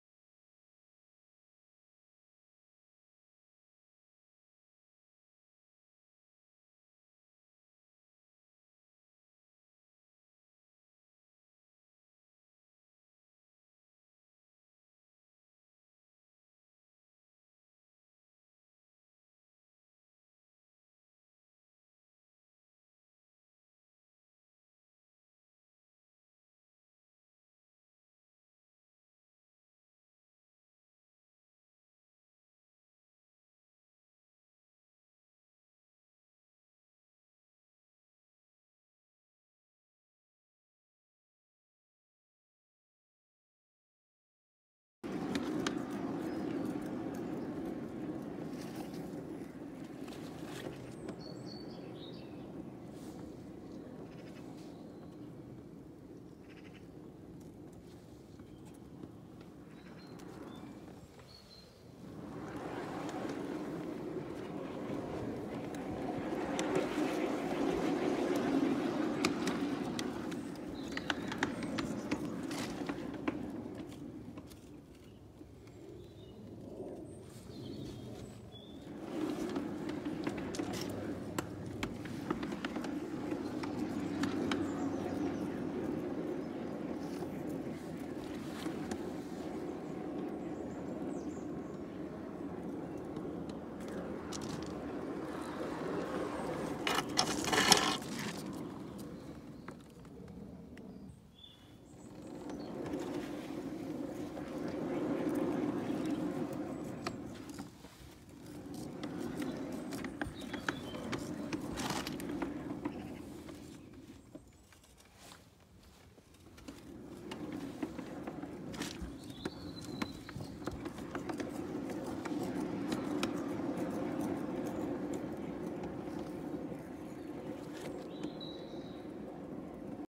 Update on my garden railway Adding a point and siding 5 inch gauge
Short video on a little bit more I’ve done on the 5 inch gauge garden railway
adding a point and siding
still plenty to do and dig